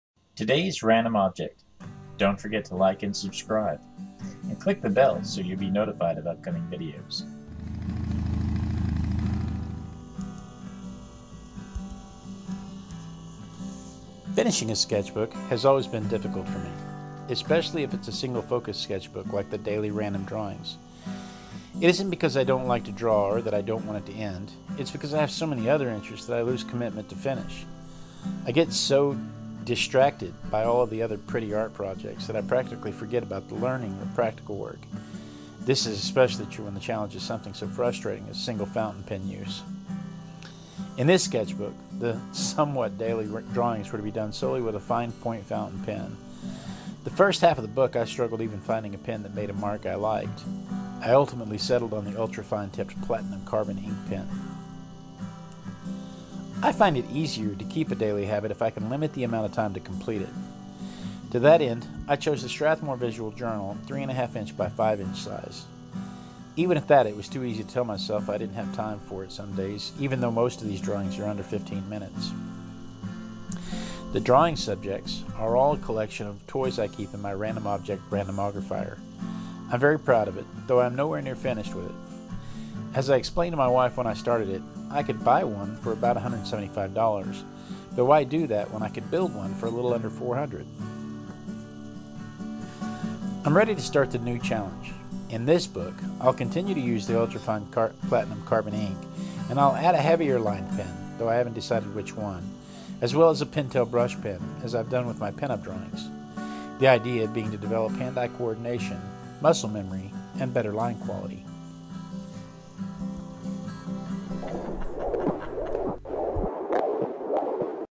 Daily Drawing Fountain Pen Sketchbook Flipthrough
Ooh ya'll I finished that little sketchbook! I really struggled at first getting a handle on using only a single fountain pen with no base pencil drawing and no other pens or markers to fill with. I got frustrated and had to put it aside a few times but here it is finally complete!